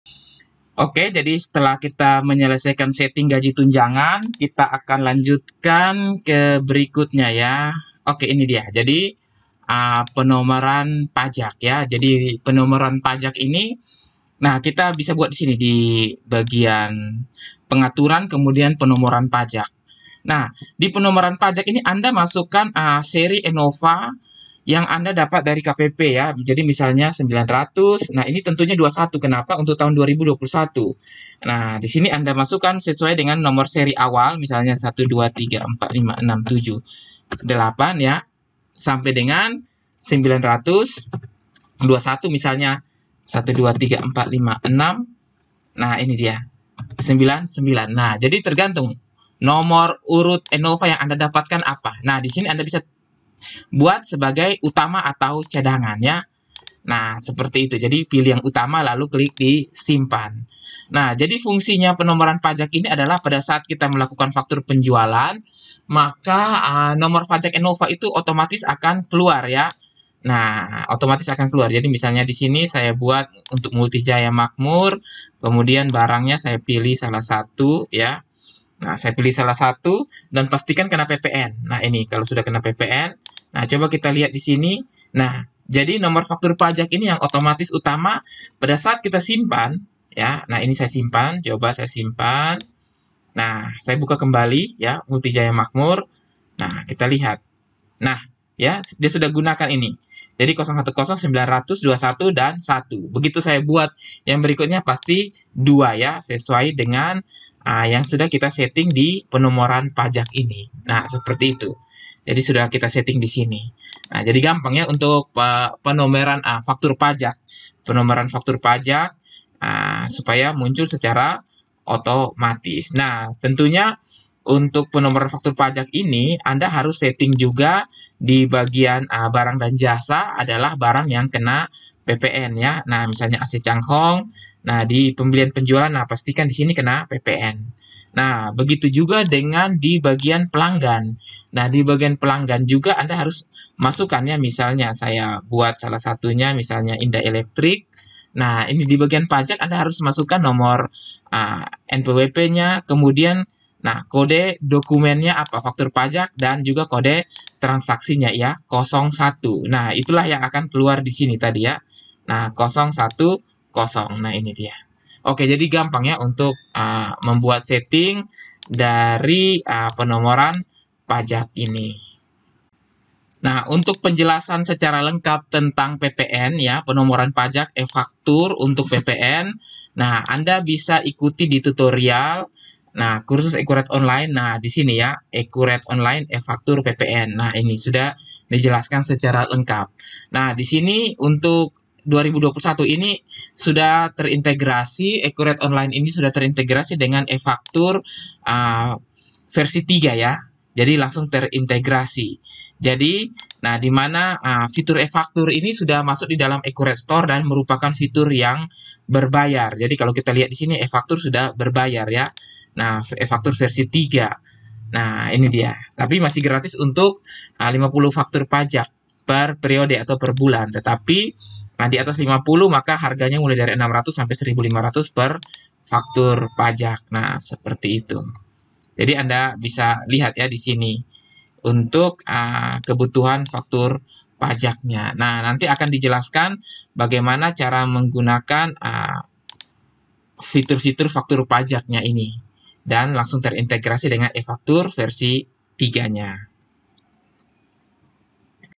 13.Setting Nomor Faktur Pajak PPN
Untuk anda yang bidang usahanya sudah PKP maka penting untuk setting awal Nomor seri faktur pajak di dalam database accurate online tahun 2021 baru kemudian mulai melakukan kegiatan penjualan. Simak video tutorial berikut ini.